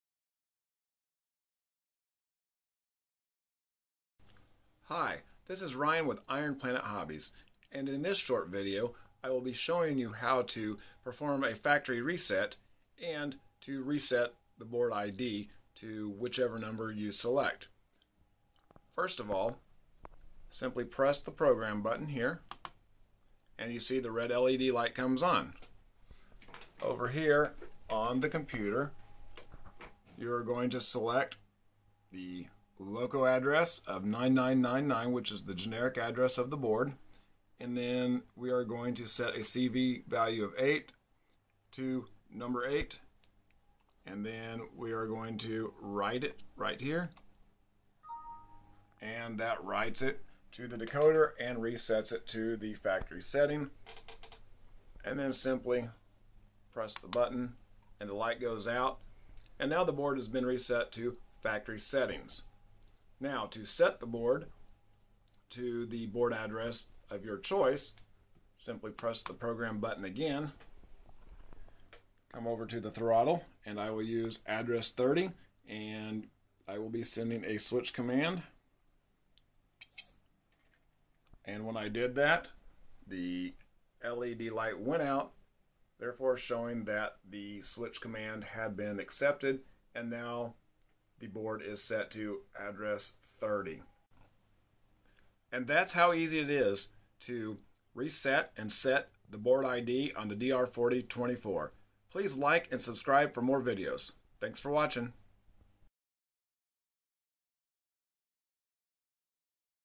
DR4024 Setup - Part 1 - Factory Reset & Board ID
This video shows how to perform a factory reset and then set the board ID on the Digikeijs DR4024 Servo Decoder.